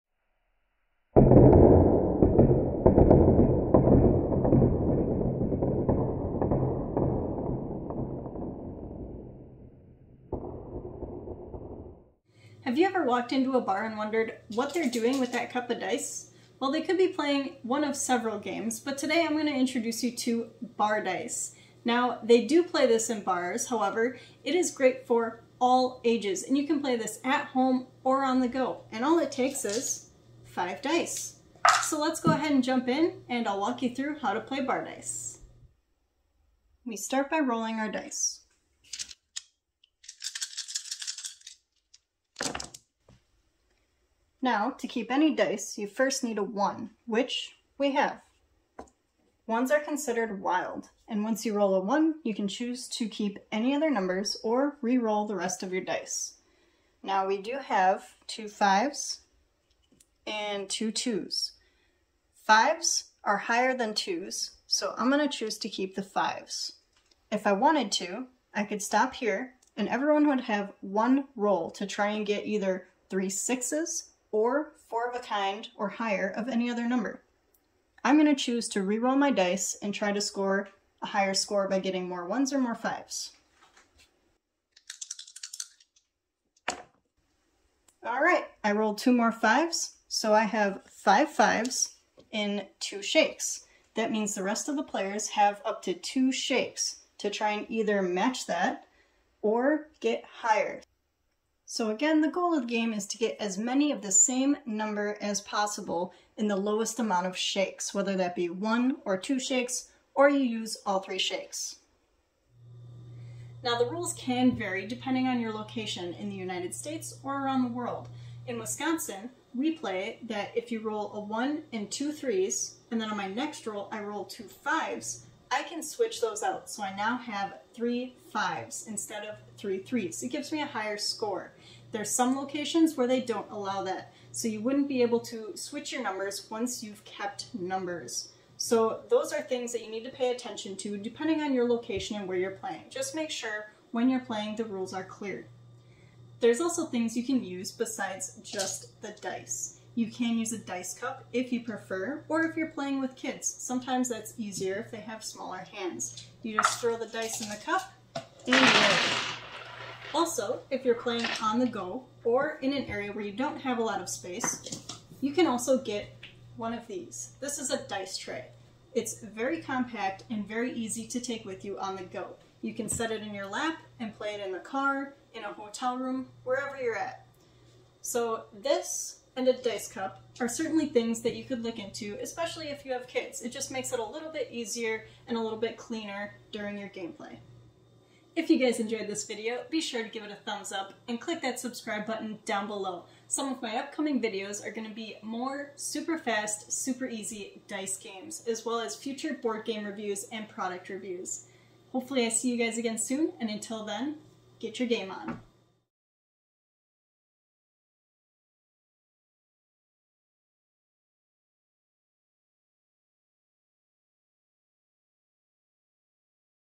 How Do You Play Bar Dice - Quick And Easy Dice Games
Are you wondering how to play bar dice, or just looking for a quick and easy dice game to play? Well stick around to learn how to play in under 2 minutes!